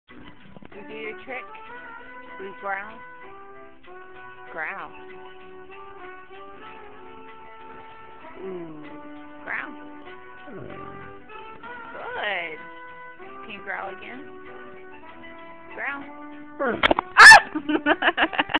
Marshmallow the Great Pyrenees
Marshmallow hates being asked to do things more than once!